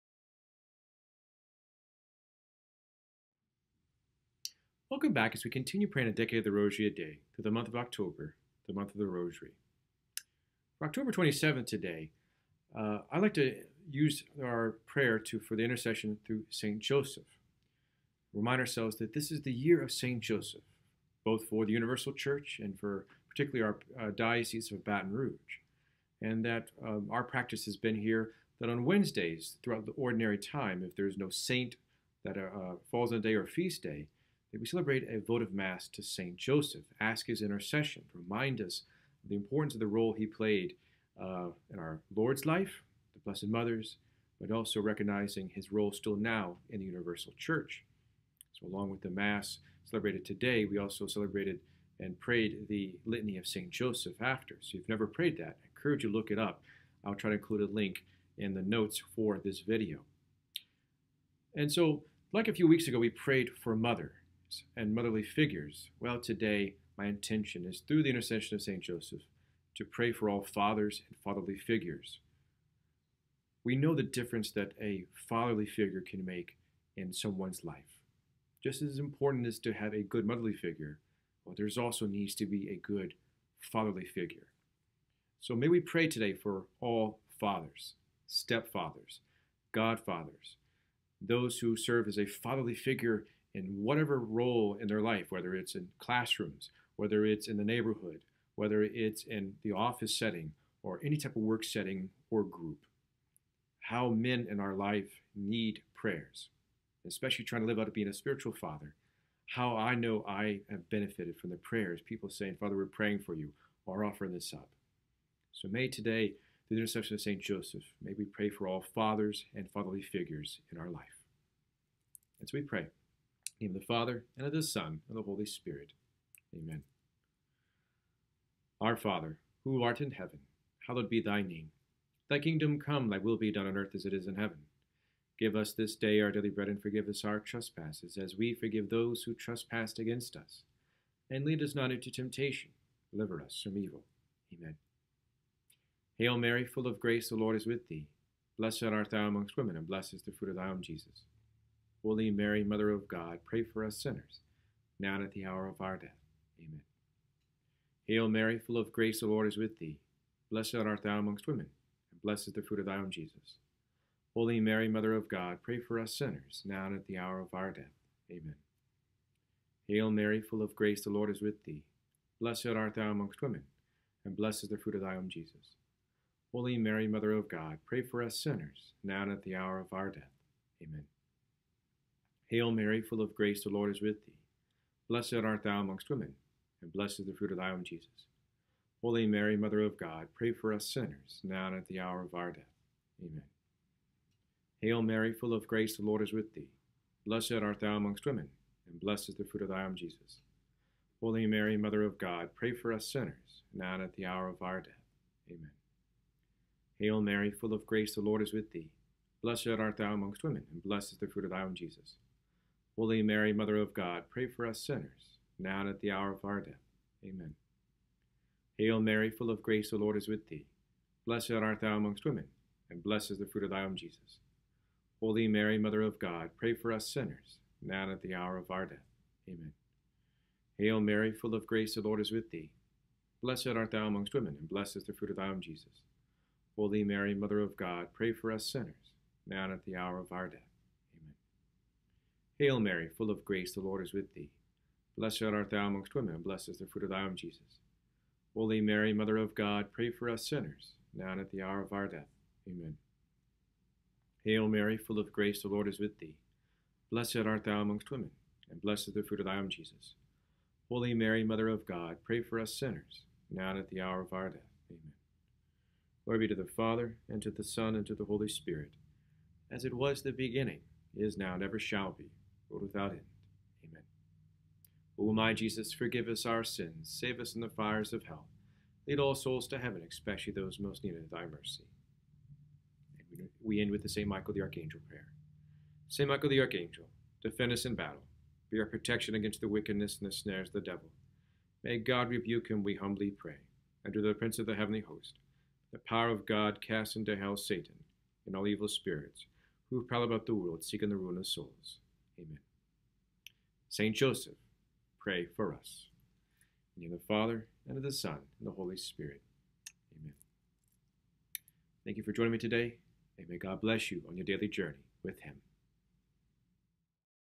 A Decade of the Rosary a Day - October 27
Today we pray for all fathers and fatherly figures in our lives. We especially offer these prayer intentions through the intercession of St. Joseph as we continue celebrating a Year of St Joseph!